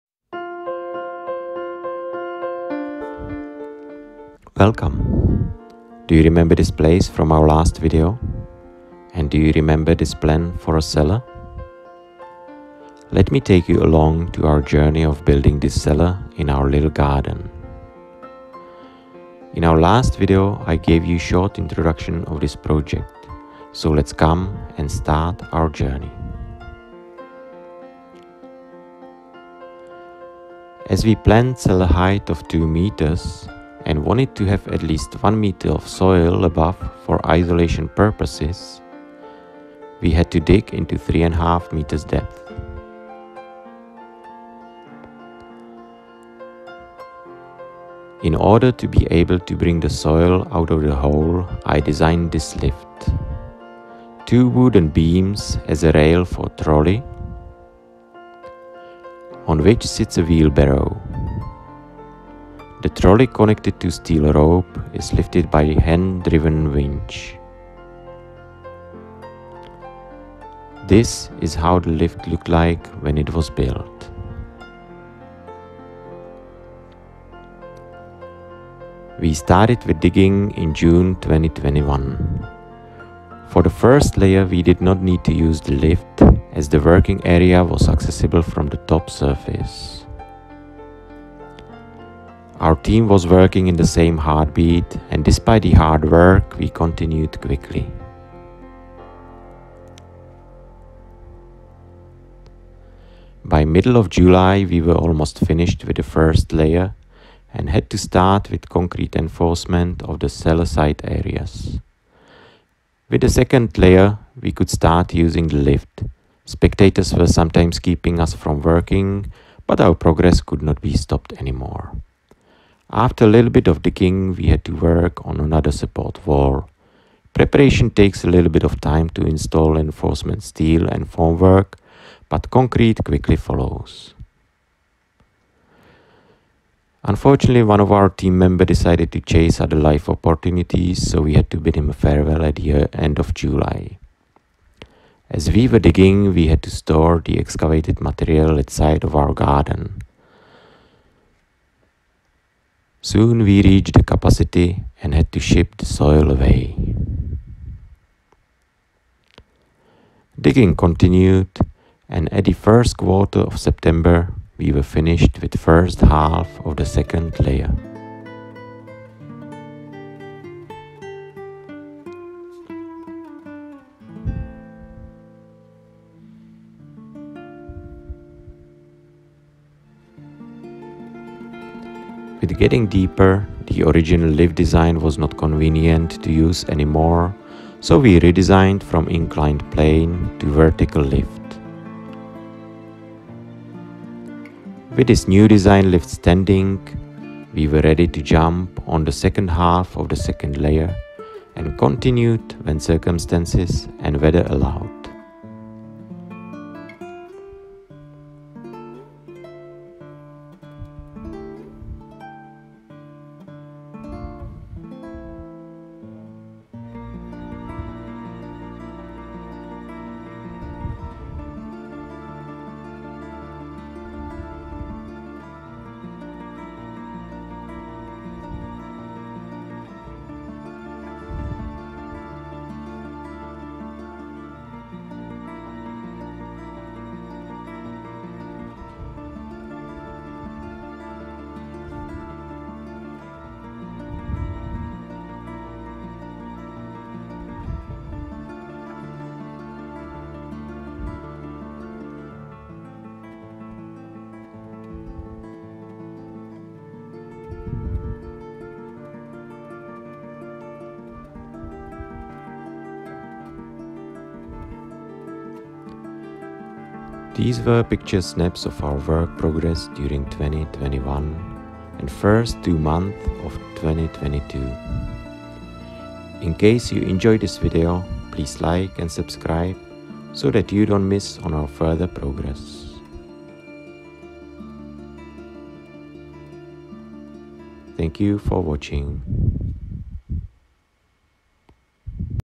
Building a root Cellar EP001
In this video, we are showing our progress from starting digging in June 2021 to end of February 2022.
この動画では、2021年6月の開始時から2022年2月までの地下室手掘り記録を紹介しています。